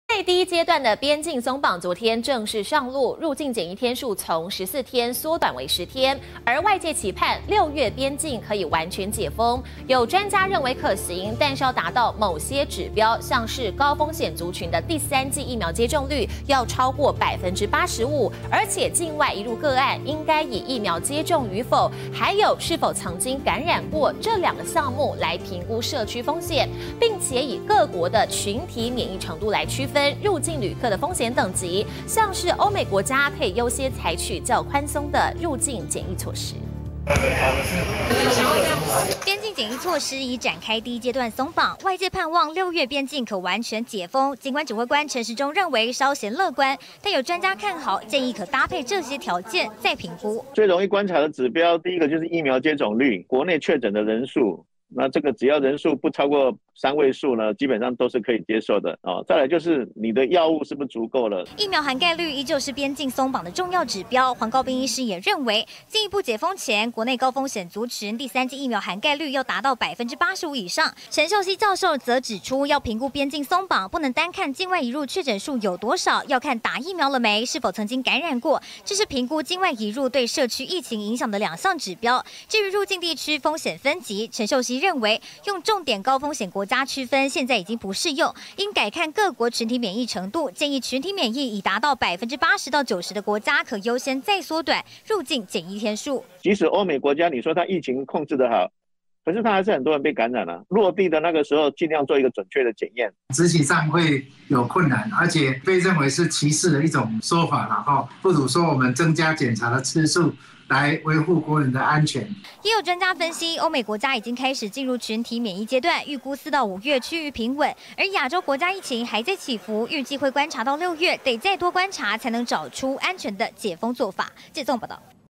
6月邊境解封?專家提議整合3大方針再評估
第一階段的邊境鬆綁 昨天（3/7）正式上路，入境檢疫天數 從14天縮短為10天，而外界期盼，六月邊境可以完全解封，有專家認為可行，但要達到某些指標，像是高風險族群的第三劑疫苗接種率，要超過85%，且境外移入個案，應該以「疫苗接種與否」、還有「是否曾經感染過」這2個項目，來評估社區風險，並以 各國的群體免疫程度，來區分入境旅客的風險等級，像是歐美國家，可優先採取較寬鬆的入境檢疫措施。